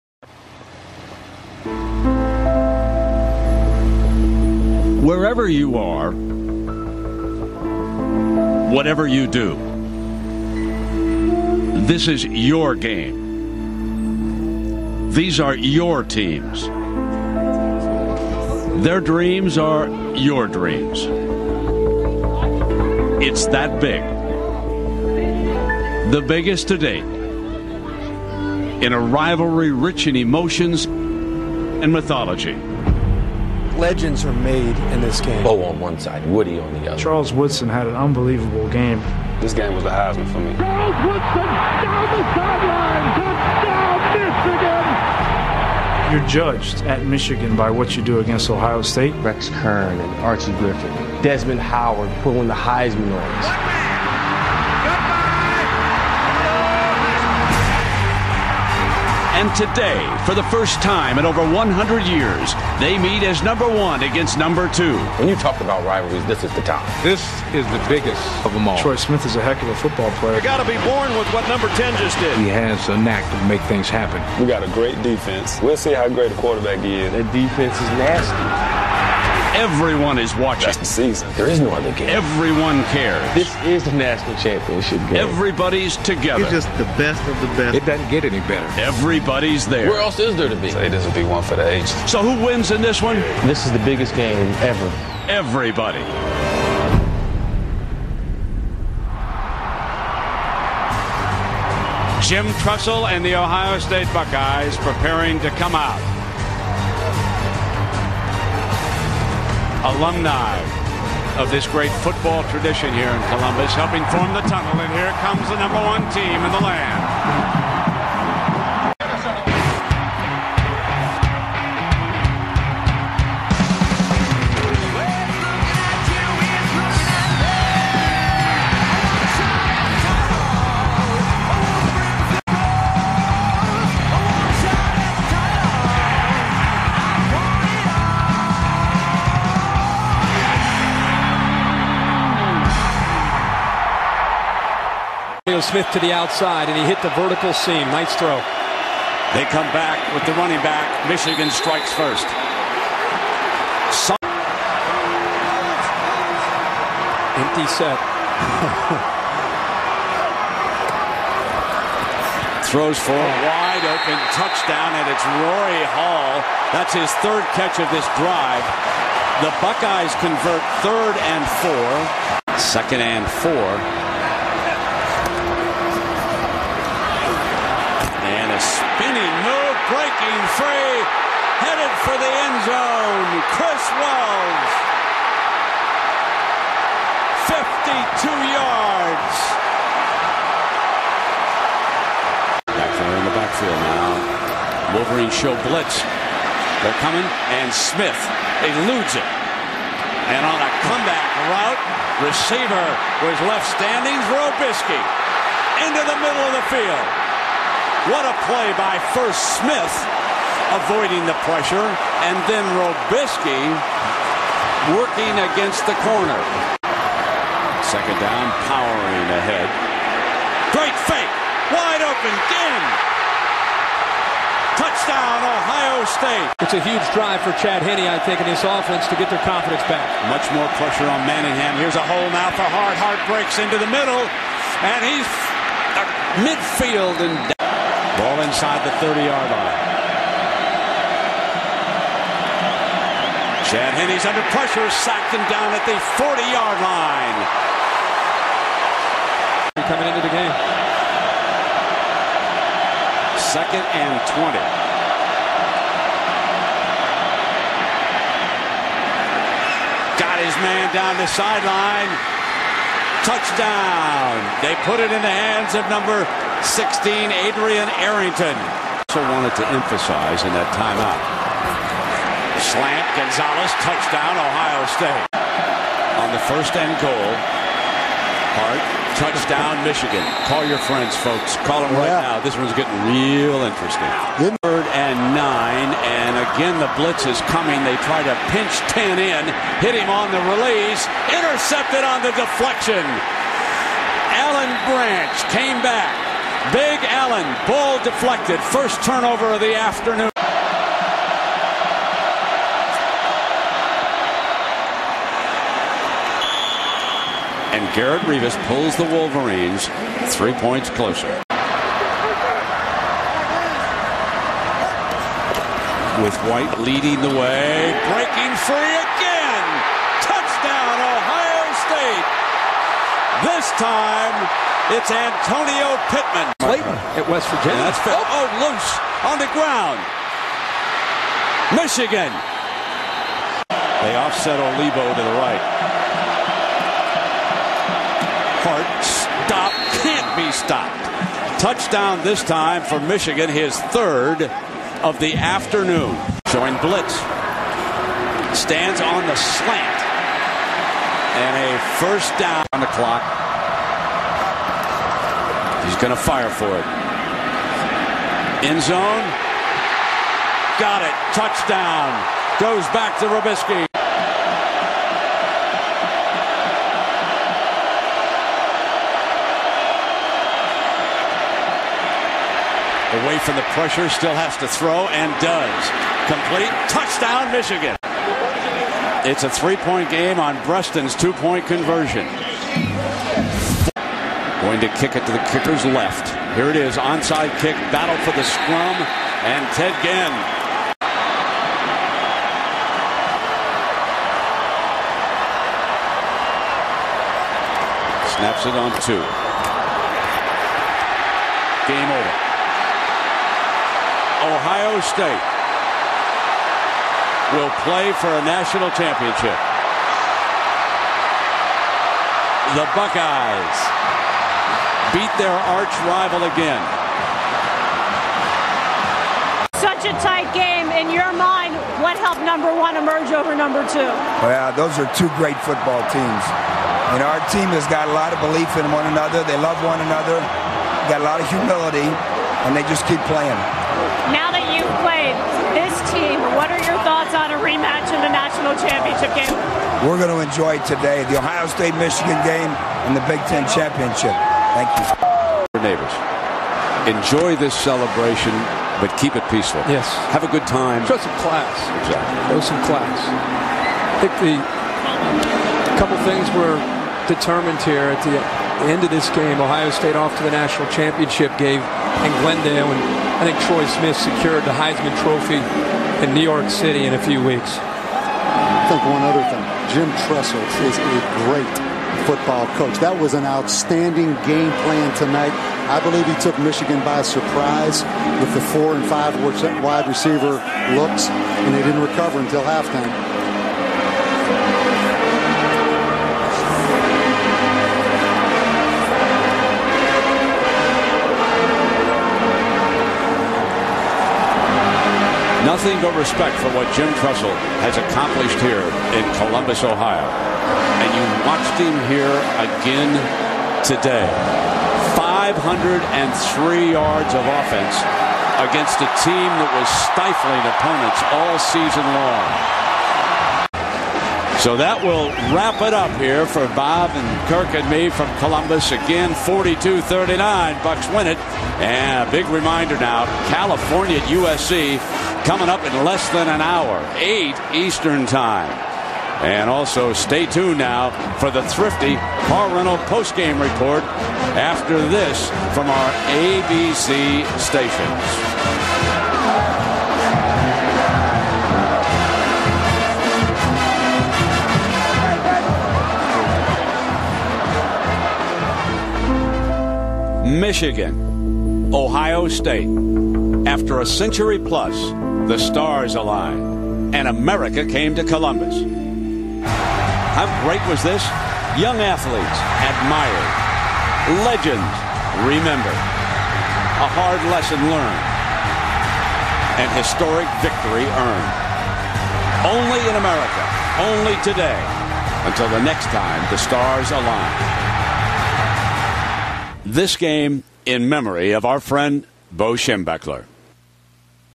2006 2Michigan @ 1Ohio State: Judgement day
this is a highlight film of the judgment day game between the #2 Michigan wolverines and the #1 Ohio State Buckeys in what is the greatest rivalry in college football. This contains the original broadcast as sound, it ain't a cheesy highlight film with some music background , its the actual broadcast. i hope hope fans of both can enjoy it.

personally i love the OSU

Go Buckeyes!